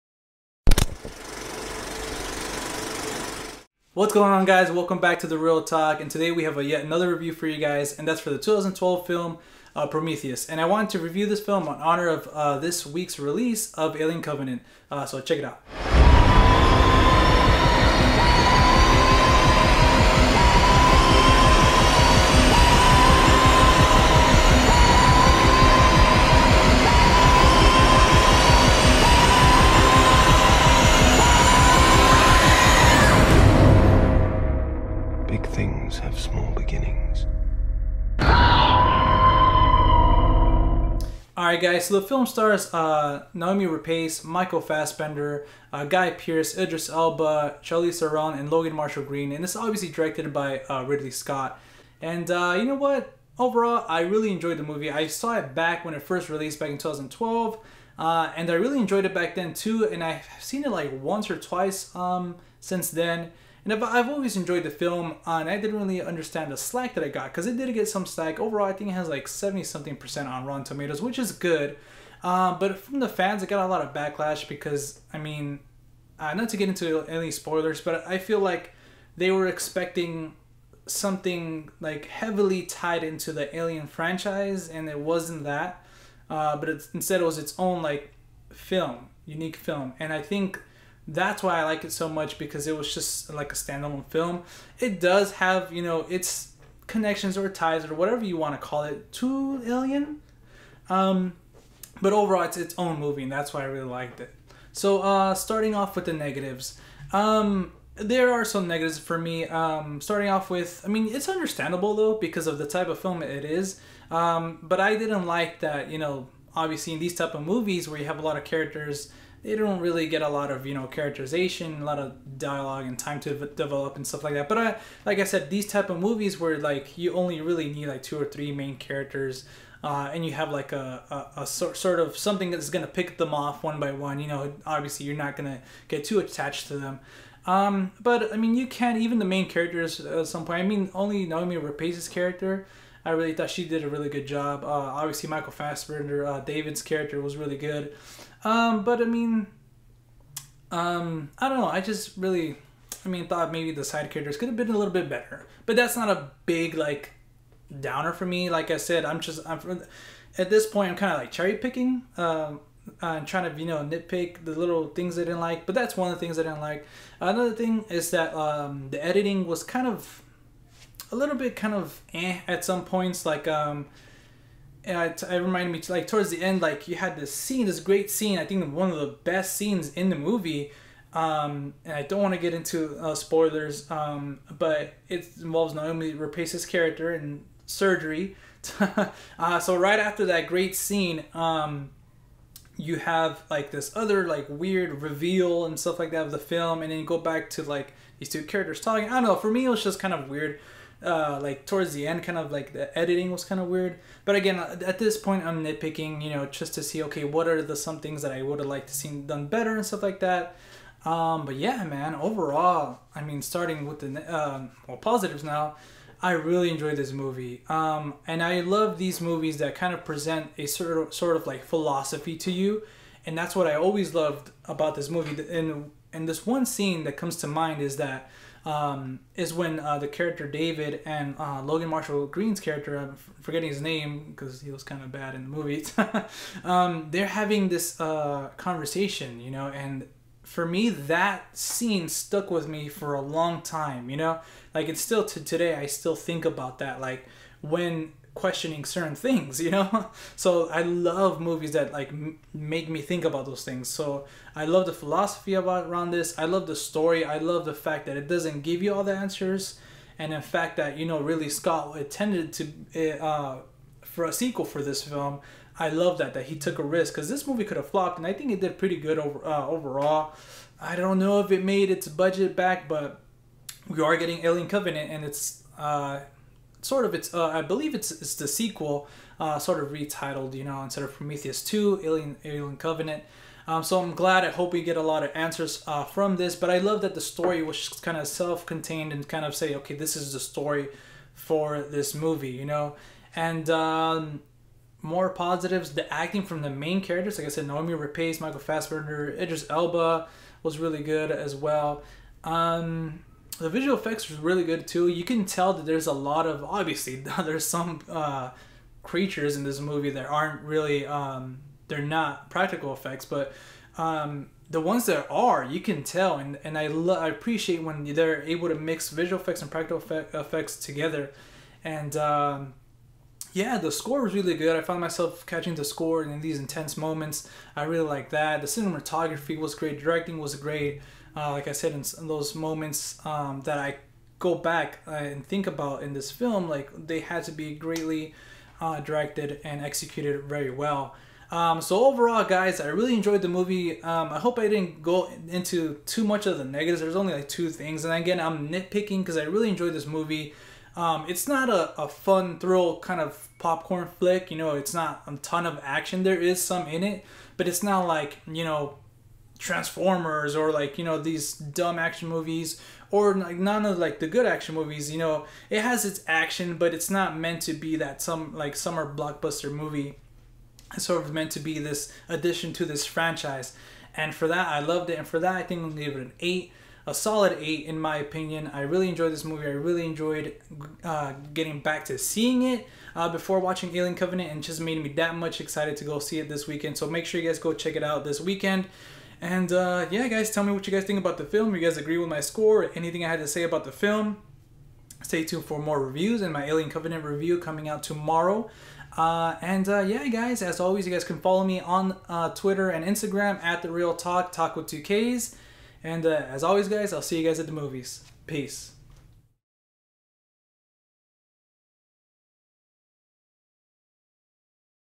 Prometheus Movie Review - The Reel Talk
In honer of Alien: Covenant being released in theaters tomorrow I reviewed the 2012 film Prometheus. The film is directed by Ridley Scott, and stars, Noomi Rapace, Michael Fassbender, Guy Pearce, Idris Elba, Logan Marshall-Green, and Charlize Theron.